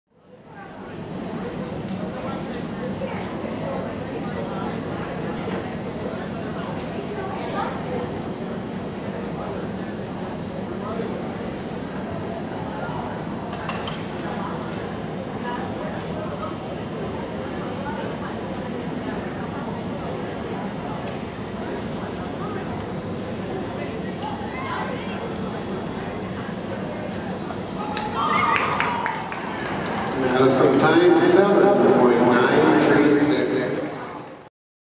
Pistol Elvis 4H Key Race
Pistol Elvis finishing third in the key race at the Minnesota 4H state show Sept. 15, 2012.